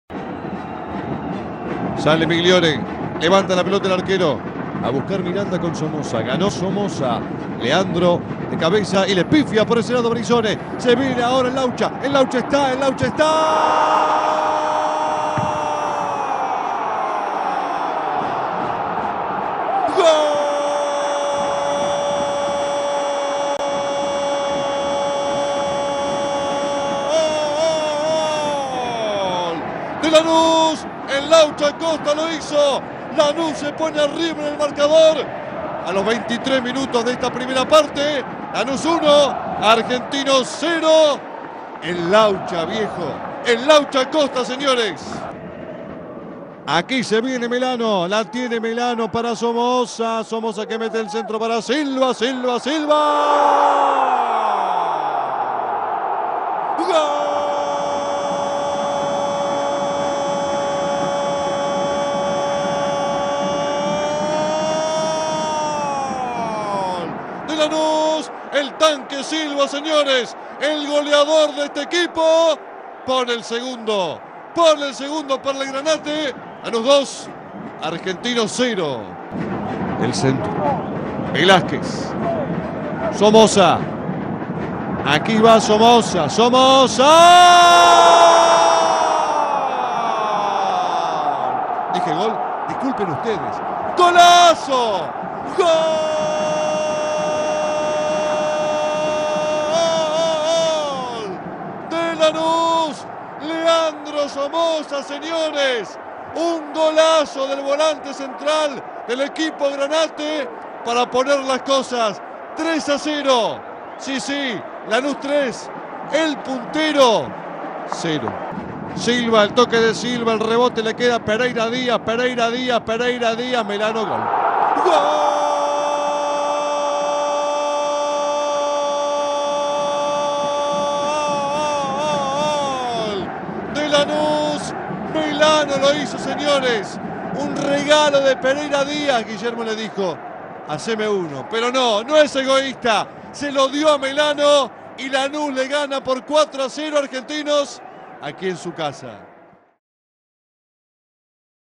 LANUS 4 ARGENTINOS 0 TORNEO INICIAL
Con goles de Acosta, Silva, Somoza y Melano, el "Granate" derrotaron a "Los Bichitos Colorados" por 4 a 0 por la séptima fecha.